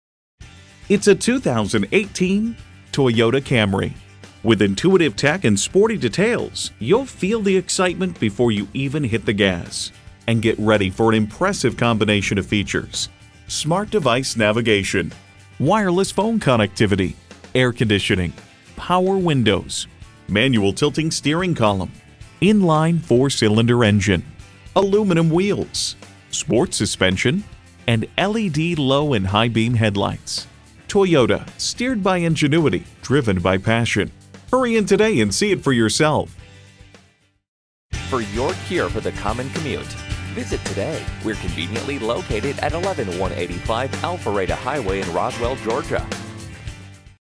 Used 2018 Toyota Camry Roswell GA Atlanta, GA 306300A - SOLD
Year: 2018
Make: Toyota
Model: Camry
Trim: SE
Engine: 2.5L I4 DOHC 16V
Transmission: 8-Speed Automatic
Color: White
Interior: Black
Mileage: 76359
Stock #: 306300A
VIN: JTNB11HK2J3022806
Recent Arrival! CARFAX One-Owner. Clean CARFAX.brbrBlack w/Perforated Leather Seat Trim, ABS brakes, Active Cruise Control, Electronic Stability Control, Illuminated entry, Low tire pressure warning, Remote keyless entry, Traction control. Super White 2018 Toyota Camry SE FWD 28/39 City/Highway MPG

Address:
11185 Alpharetta Hwy